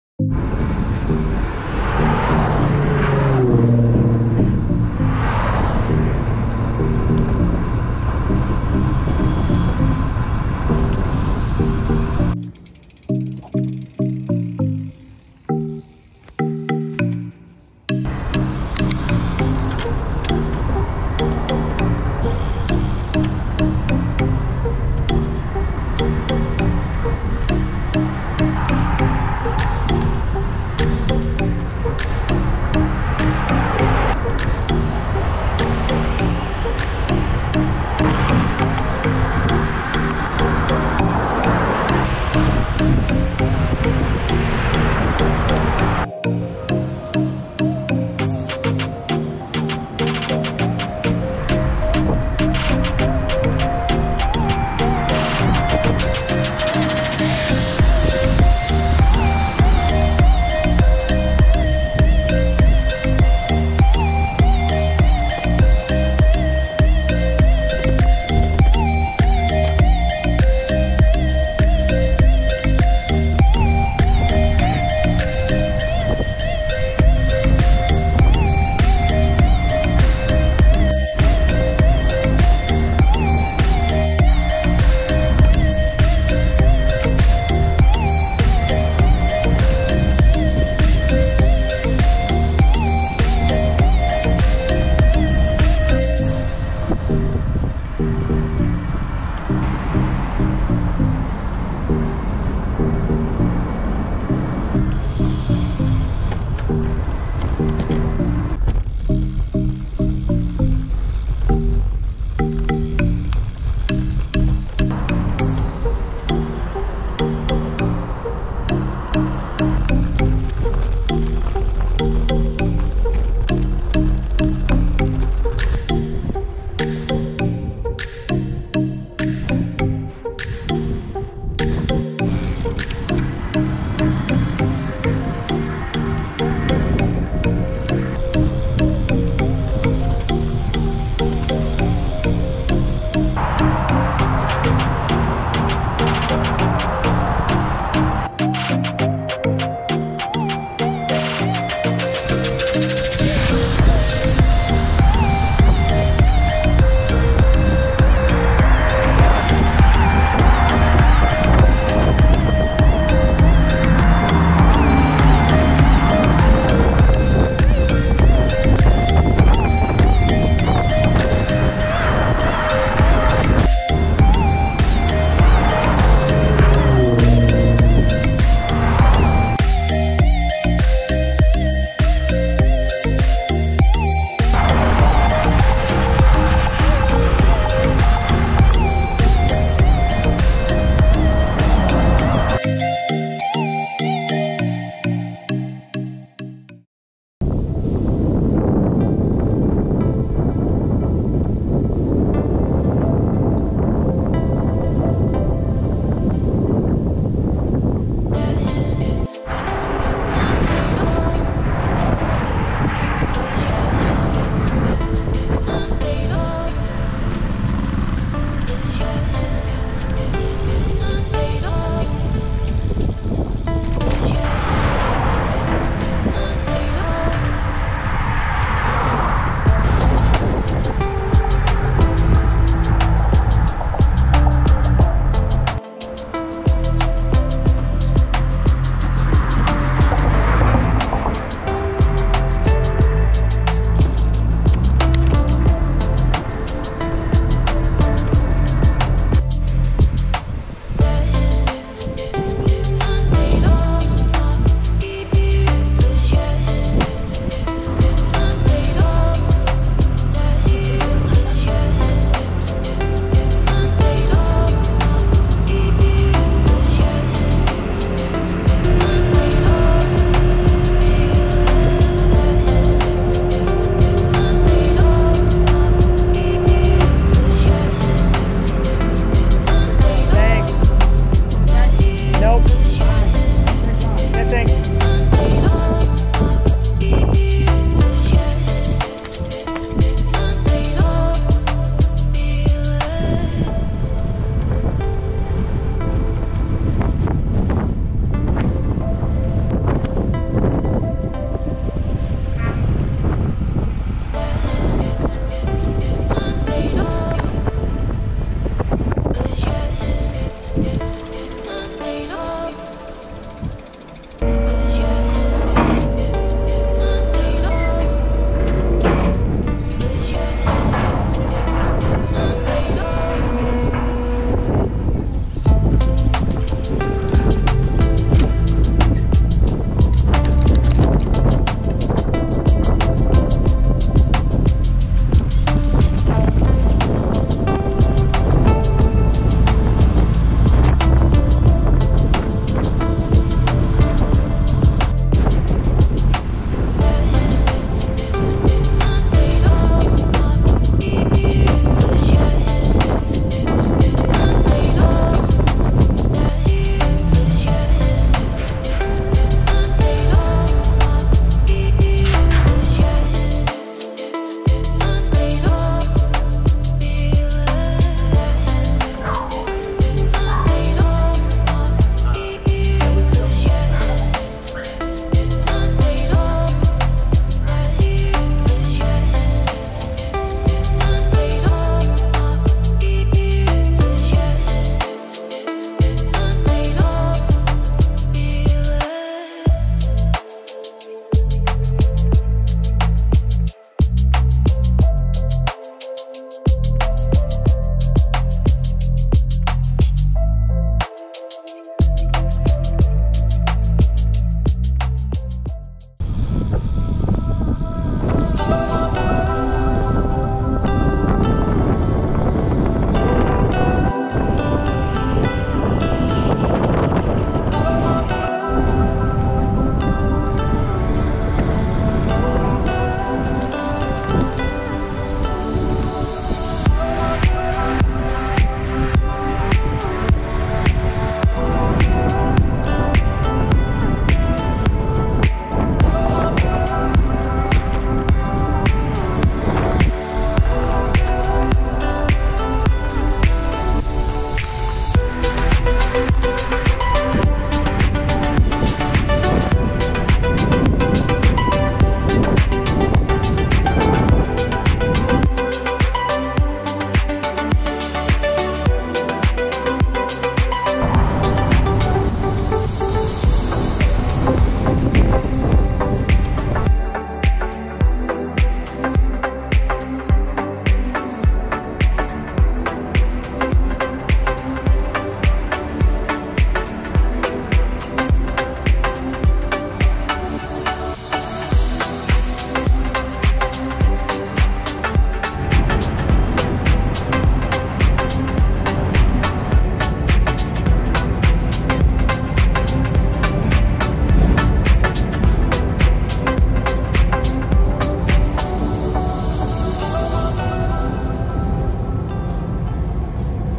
Fremont to Sacramento Bike Tour
Rode my electric bike over May 1-2, 2021, to get from Fremont to Sacramento. Day 1 was 75 miles, day 2 was 55 miles.  Each day I had to recharge my bike mid-day, as well as overnight.  Camped at Brannan Island SRA, which takes bicyclists without reservation.  Returned to Bay Area via Amtrak.